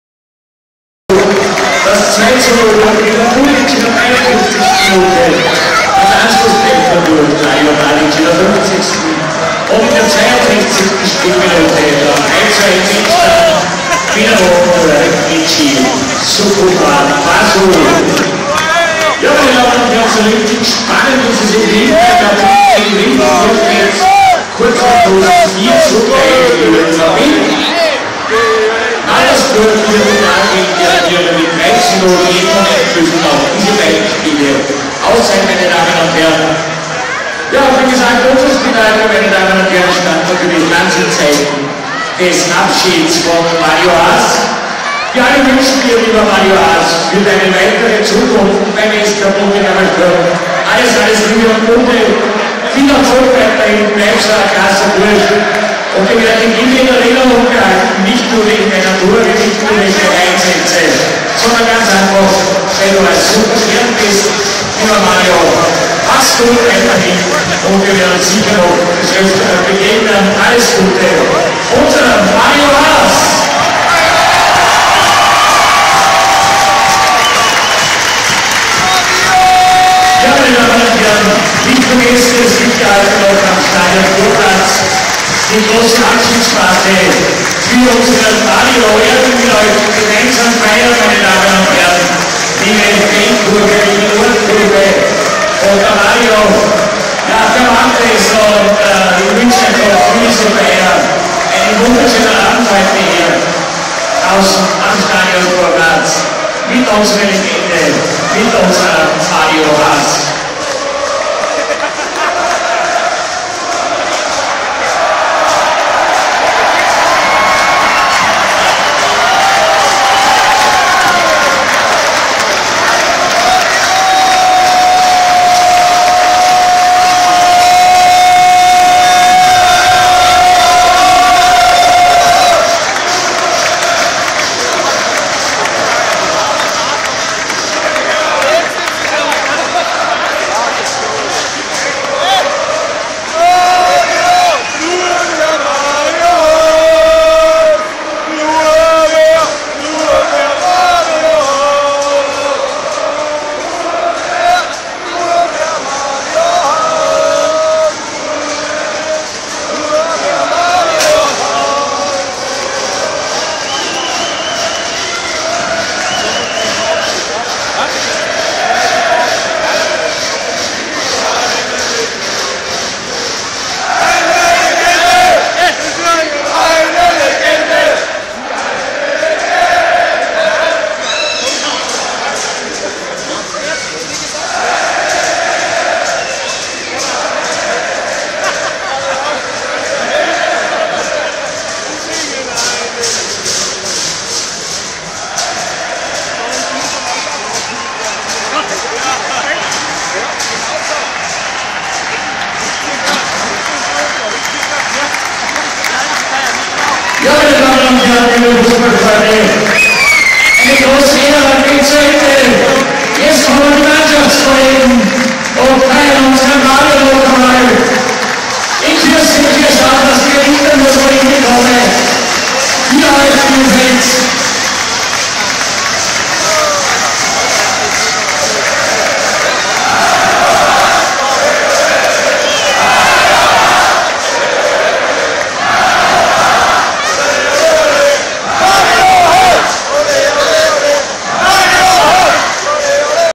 Verabschiedung von Sturm Legende Mario Haas
Der Bomber wird vom Publikum verabschiedet.